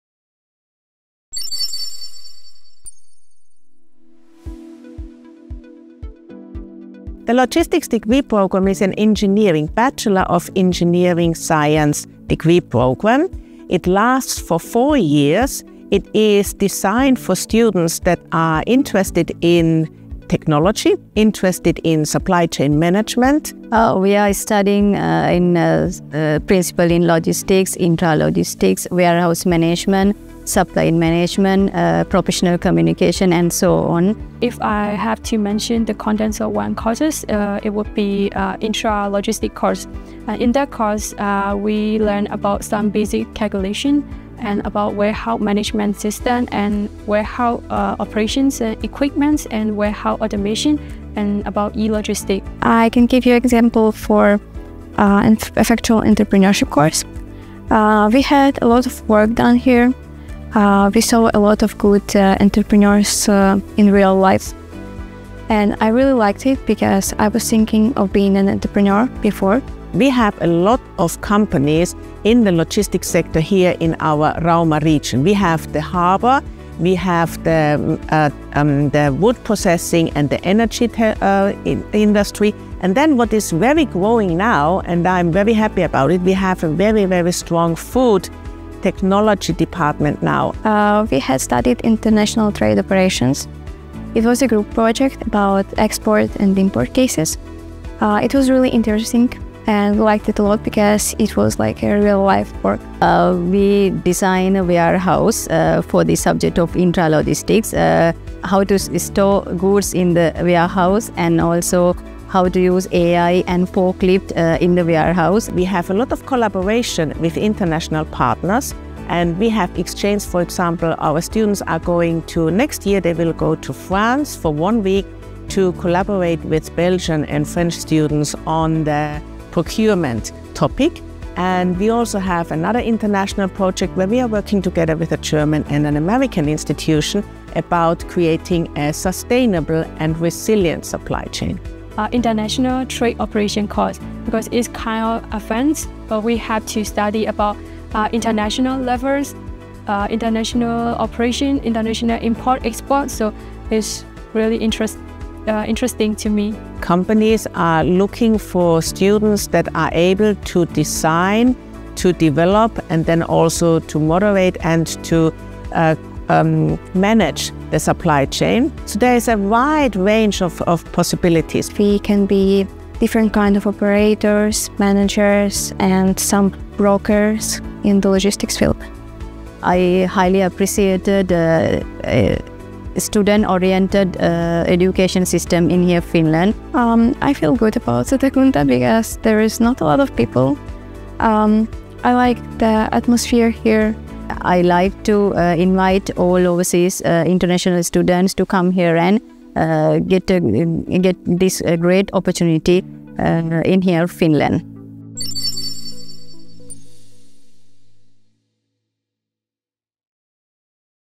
Logistics – Study bachelor's degree in English at SAMK
Study logistics engineering including external and outbound logistics. Your studies also include transportation technology and distribution management. During the studies you will gain an understanding of logistics management.

Jayamini, Yeing, and Kseniia, Mechatronics students, share their experiences about Satakunta University of Applied Sciences. Also Head of Teaching, Daniela Tanhua, gives deeper insight about the degree programme.